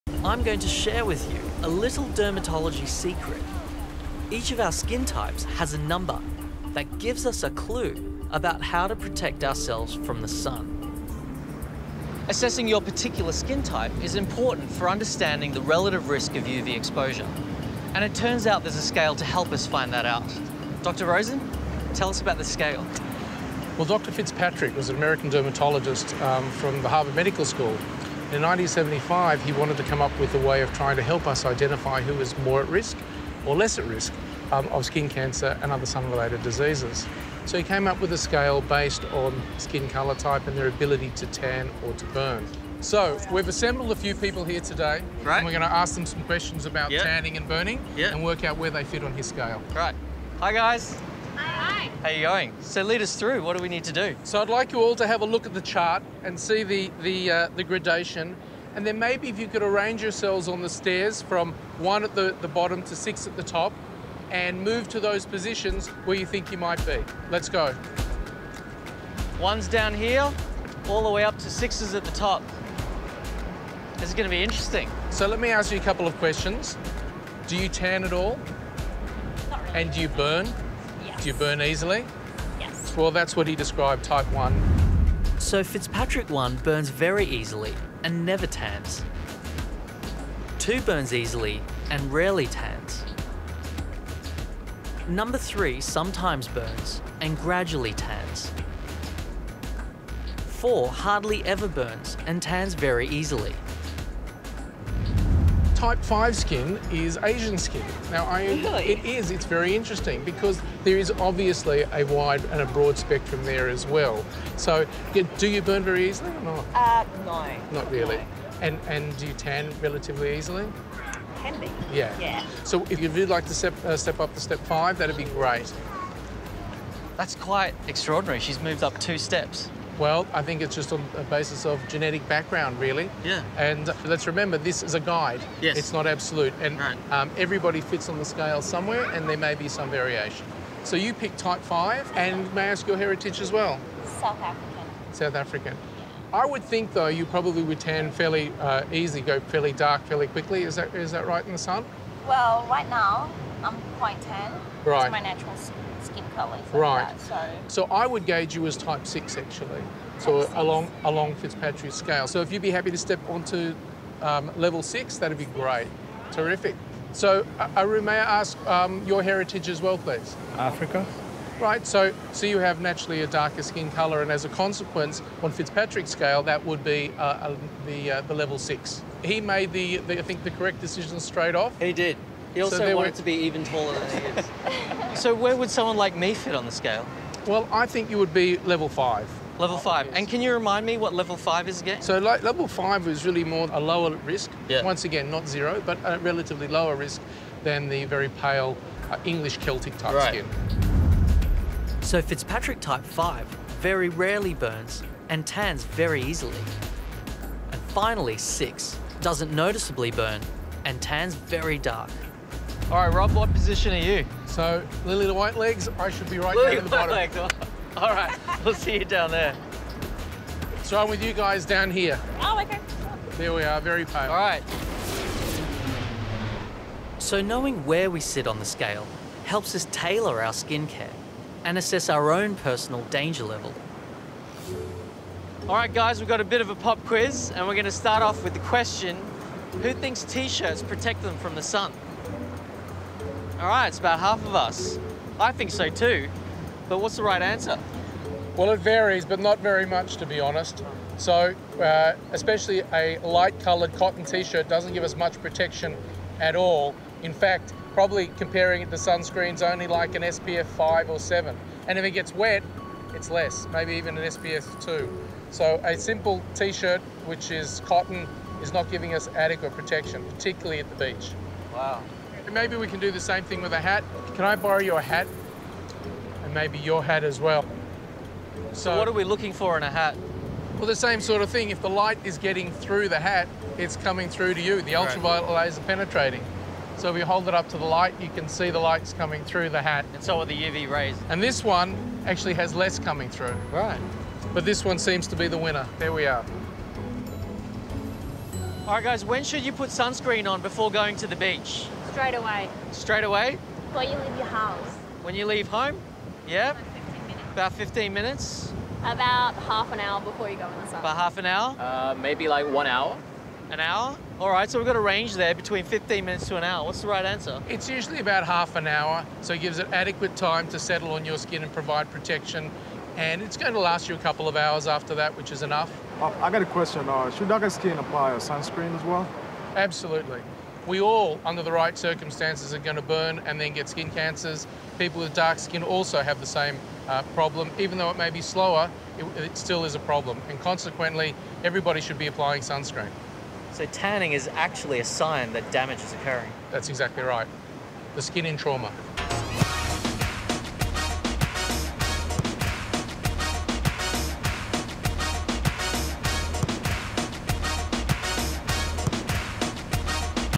What Is Your Skin Type?
Each of our skin types has a number that gives a clue about how to protect ourselves from the sun. In 1975, Dr Fitzpatrick from Harvard Medical School came up with a scale based on skin colour, type, and ability to tan or burn. Find out where your skin falls on the scale.